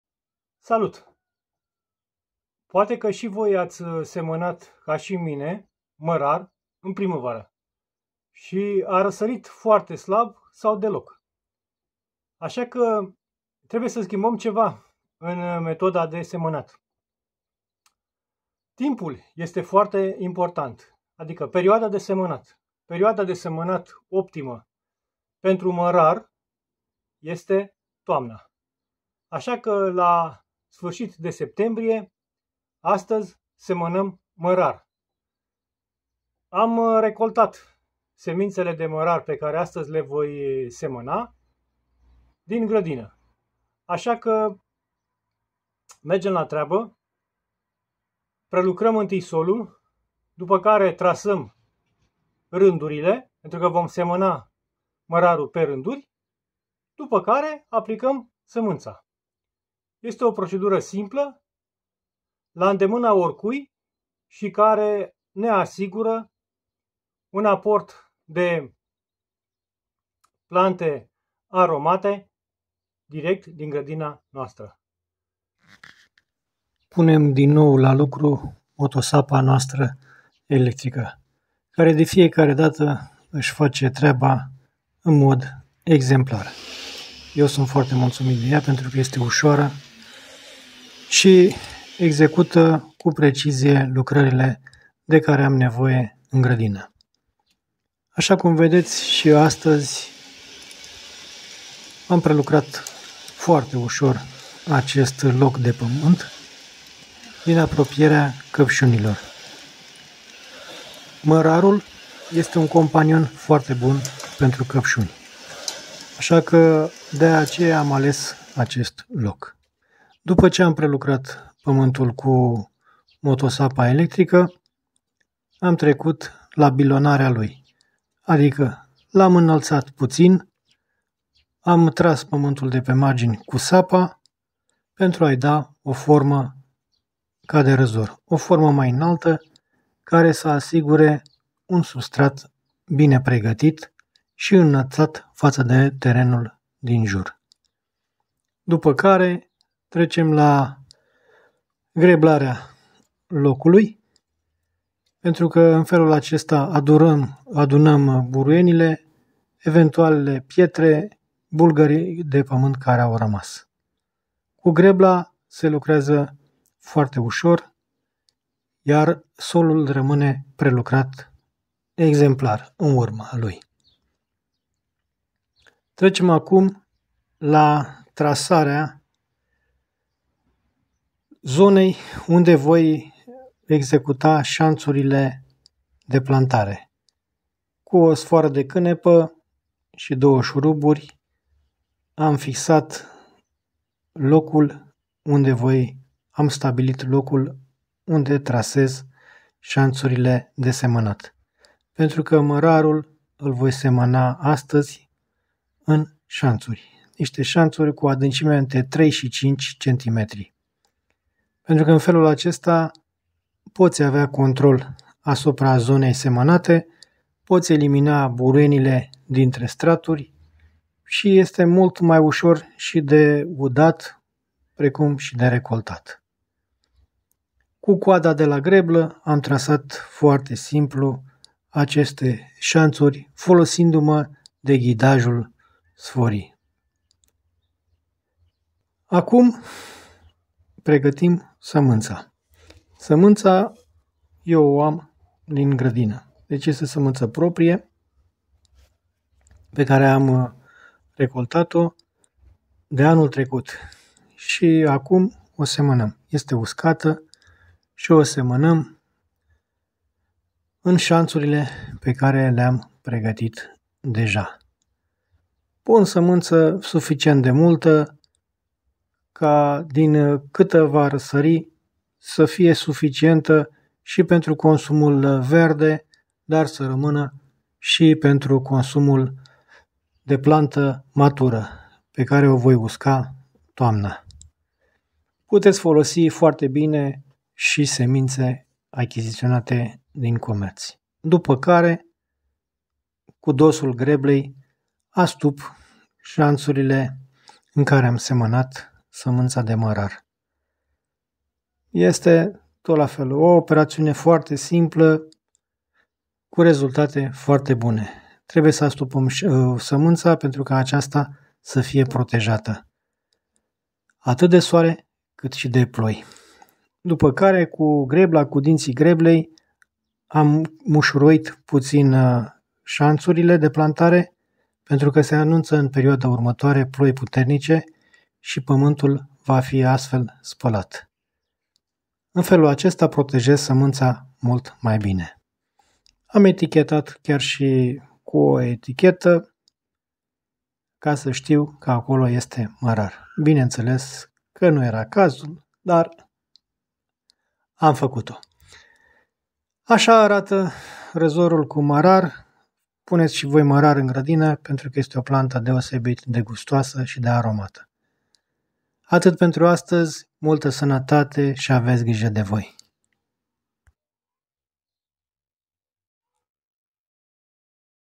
Când și cum se seamănă mărarul?
Mărarul (Anethum graveolens) este o plantă anuală, cu viață scurtă, nativă sud-vestului și centrului Asiei.